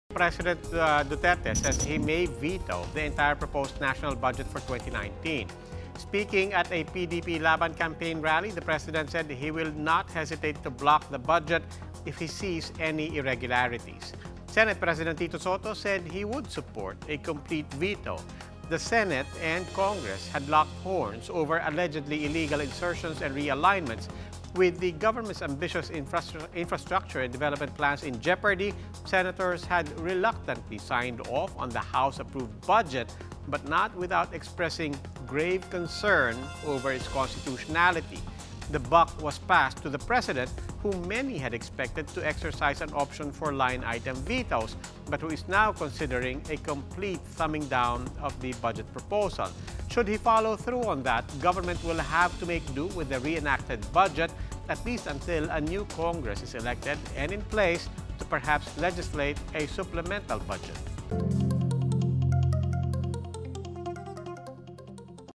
Pres. Duterte threatens to veto entire budget
Pres. Rody Duterte says he will not hesitate to block the proposed 2019 national budget should he find any irregularity.